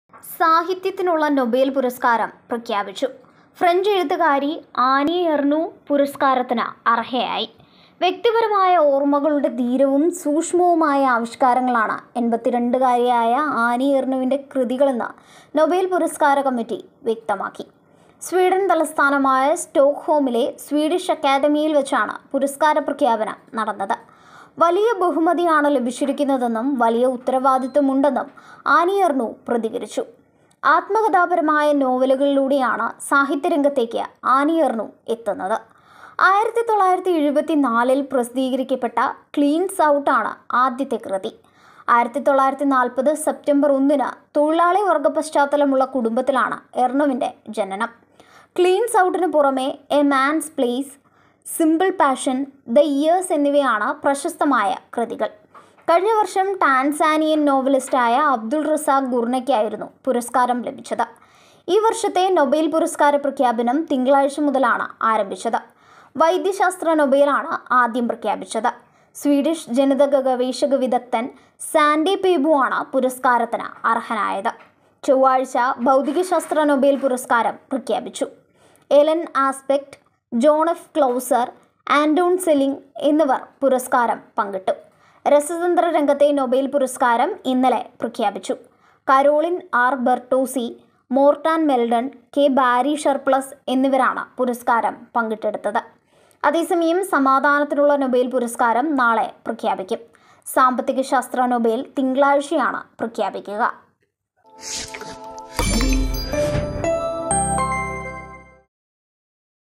സാഹിത്യത്തിനുള്ള നൊബേൽ പുരസ്കാരം ഫ്രഞ്ച് എഴുത്തുകാരി അനീ എർനുവിന്
സാഹിത്യത്തിനുള്ള നൊബേൽ പുരസ്കാരം പ്രഖ്യാപിച്ചു. ഫ്രഞ്ച് എഴുത്തുകാരി അനീ എർനു പുരസ്കാരത്തിന് അർഹയായി. വ്യക്തിപരമായ ഓര്‍മകളുടെ ധീരവും സൂക്ഷ്മവുമായ ആവിഷ്‌കാരങ്ങളാണ് 82 കാരിയായ അനീ എർനുവിൻ്റെ കൃതികളെന്ന് നൊബേൽ പുരസ്കാര കമ്മിറ്റി വ്യക്തമാക്കി. സ്വീഡൻ തലസ്ഥാനമായ സ്റ്റോക്ക്ഹോമിലെ സ്വീഡിഷ് അക്കാദമിയിൽ വെച്ചാണ് പുരസ്കാര പ്രഖ്യാപനം നടന്നത്. വലിയ ബഹുമതിയാണ് ലഭിച്ചരിക്കുന്നതെന്നും വലിയ ഉത്തരവാദിത്തുമുണ്ടെന്നും അനീ എര്‍നു പ്രതികരിച്ചു. ആത്മകഥാപരമായ നോവലുകളിലൂടെയാണ് സാഹിത്യരം​ഗത്തേക്ക് അനീ എര്‍നു എത്തുന്നത്.